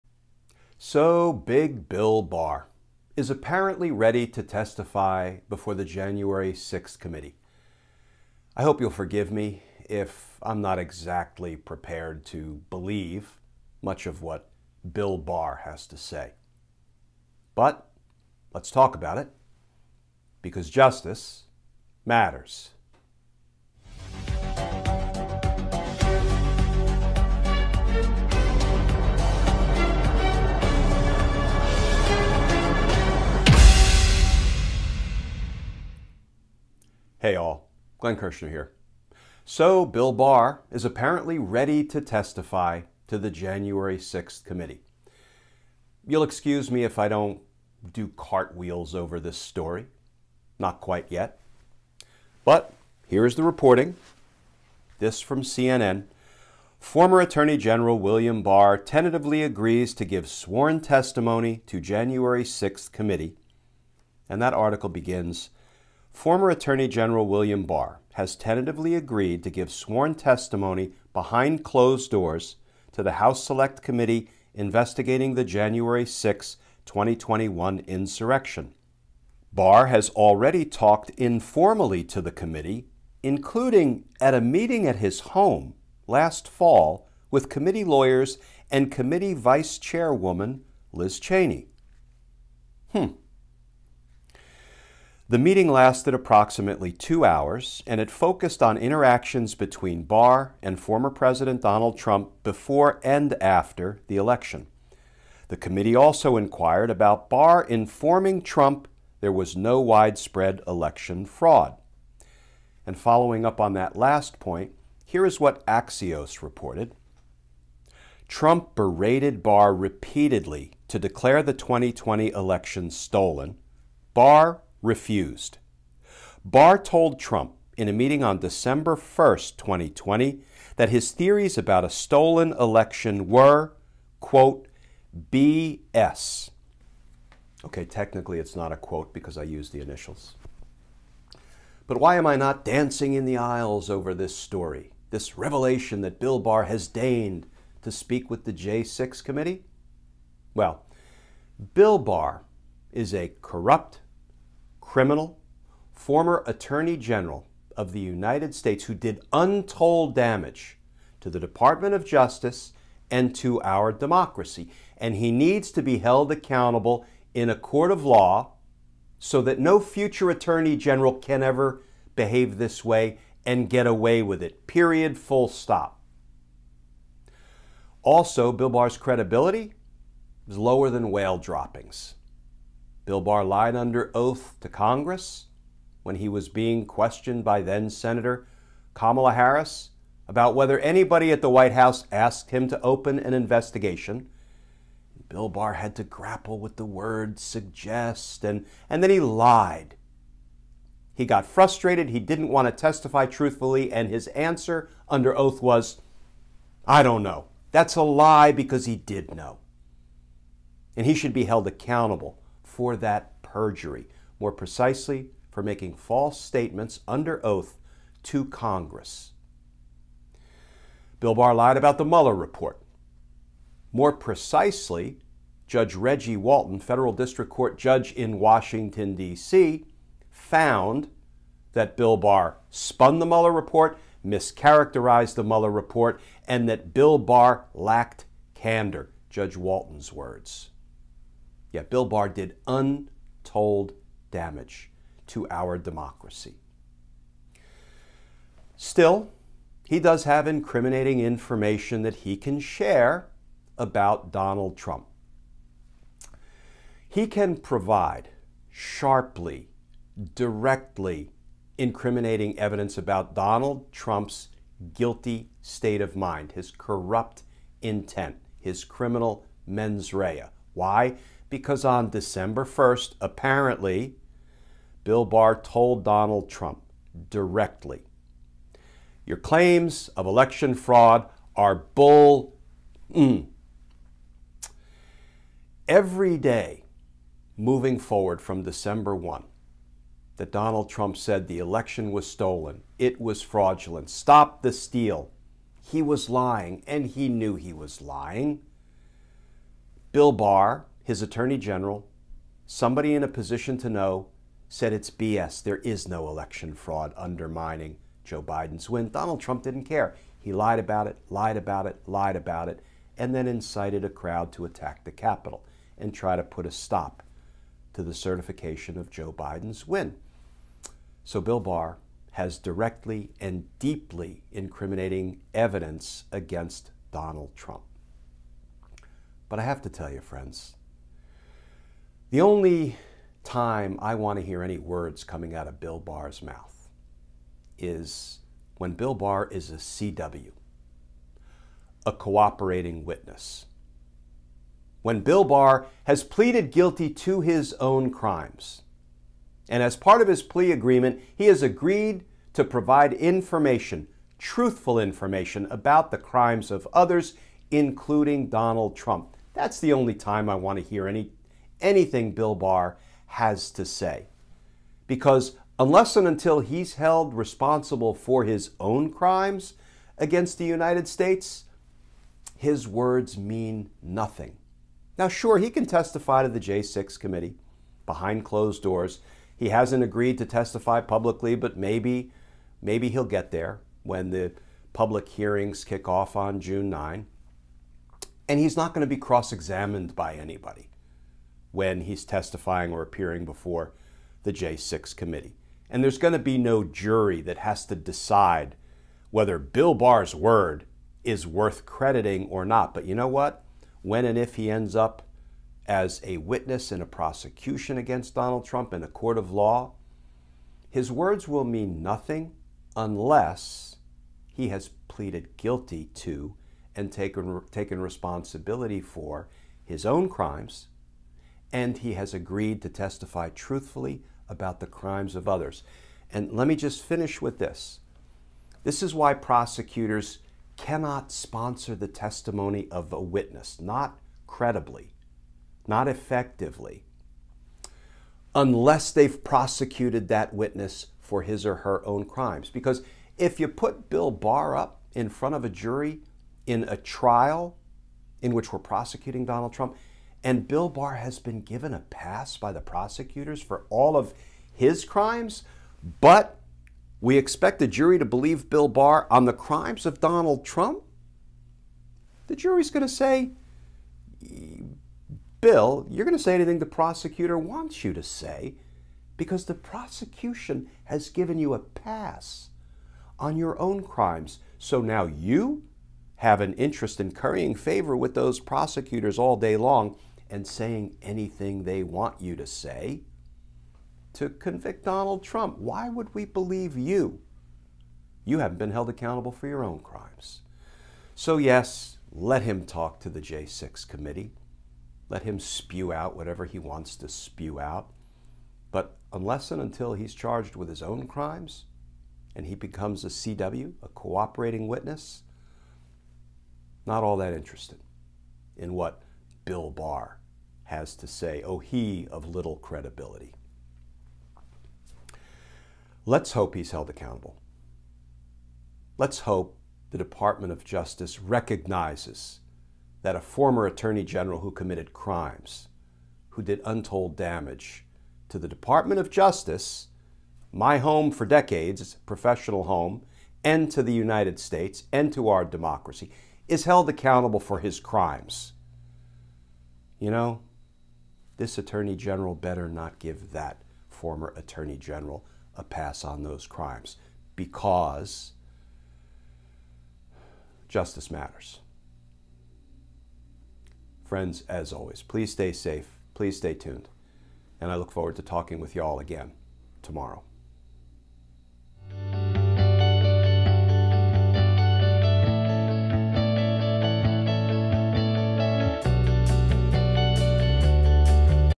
Bill Barr to testify to J6 committee about Trump's crimes. Will he be a credible witness?
Former Attorney General Bill Barr will testify to the House select committee investigating the insurrection. But why would anyone believe anything Bill Barr has to say?

There is certainly one way to improve Barr's credibility: charge him with his crimes and develop him as what prosecutors call a "cooperating witness."

This video discusses the import and challenges of Barr's upcoming congressional testimony.

My podcast, "Justice Matters with Glenn Kirschner" can be downloaded where you get your podcasts.